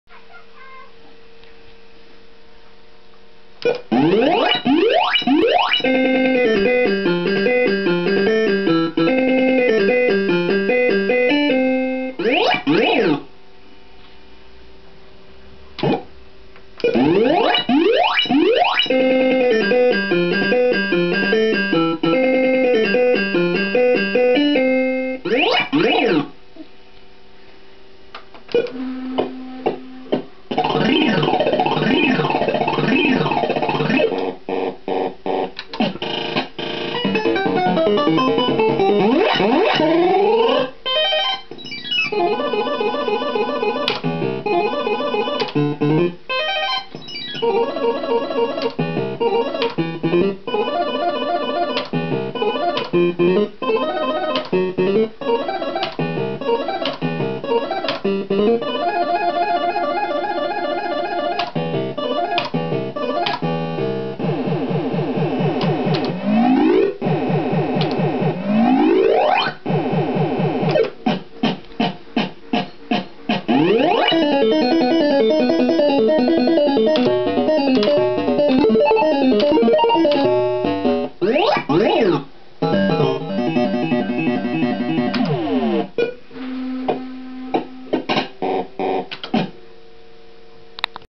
BwB Nudge Fever (MPS2) Jackpot tune
I did cheat and do this in test. In fact twice lol. So you can hear
the sweet Jackpot tune for £2 :-)